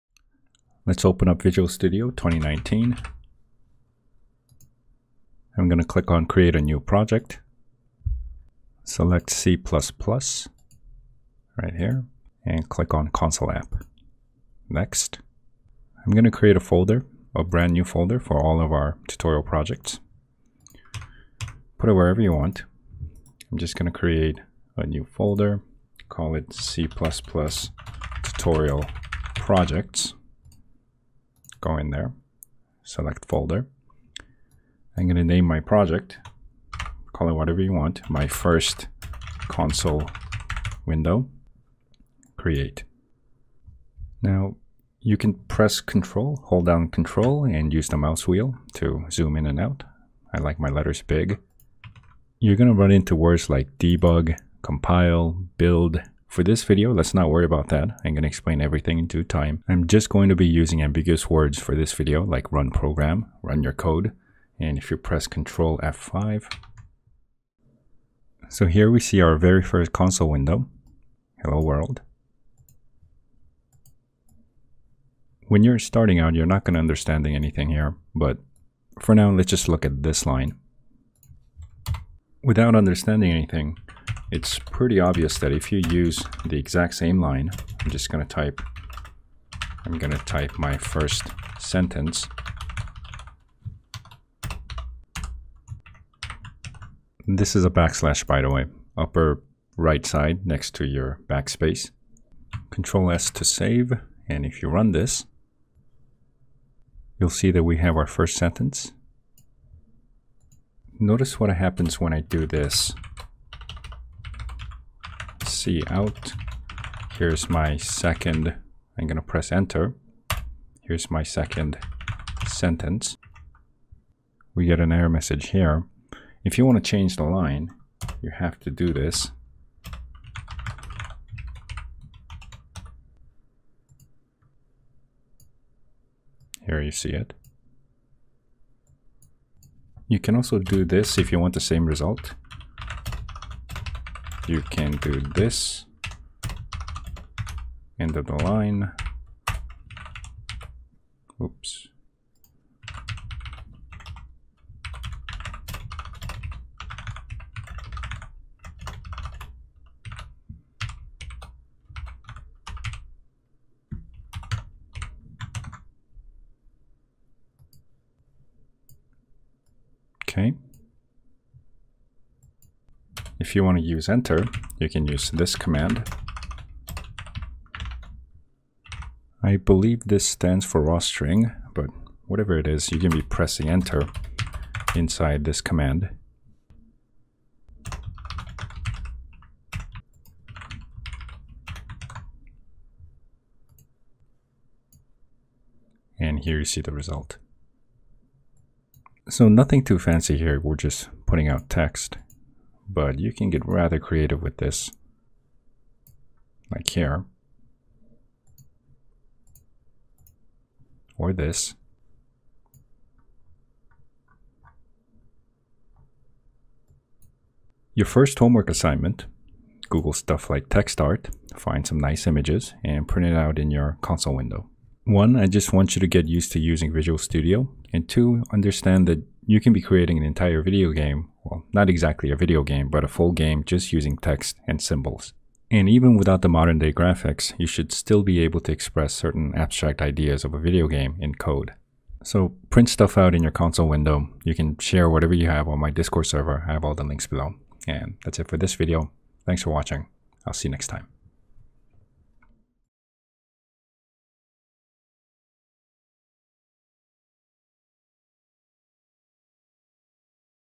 [C++ Tutorial #0.1] Your First Homework Assignment
Here's the second simplest task possible, character output, std::cout. We still haven't started learning C++ yet, but I wanted to include some absolute basics for beginners.